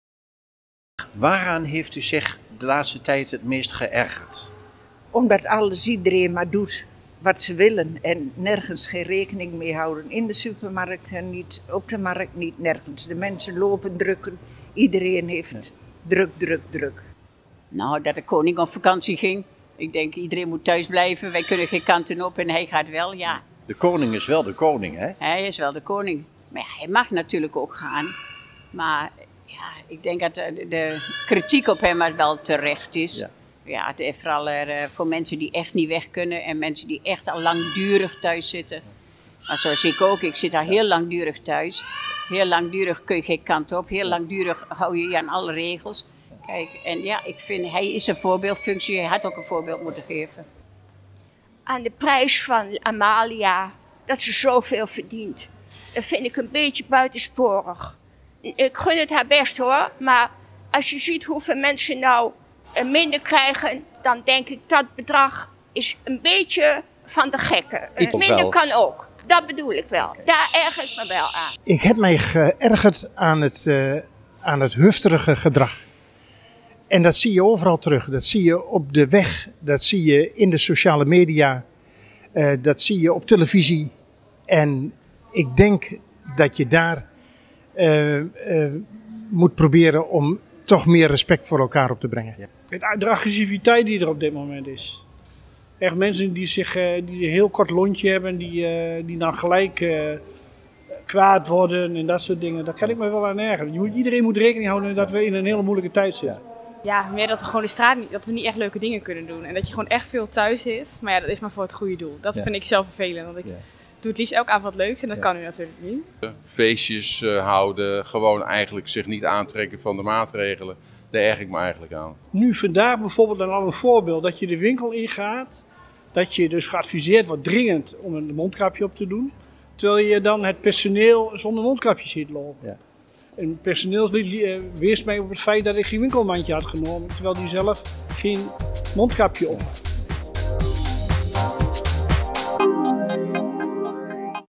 Stroatproat: Ergernissen
Waaraan heeft u zich de laatste tijd het meest geërgerd? Deze vraag werd aan passanten in de galerij gesteld. Ergernissen zijn van alle tijden, maar het is alsof de coronatijd de irritatie onder mensen juist aanwakkert.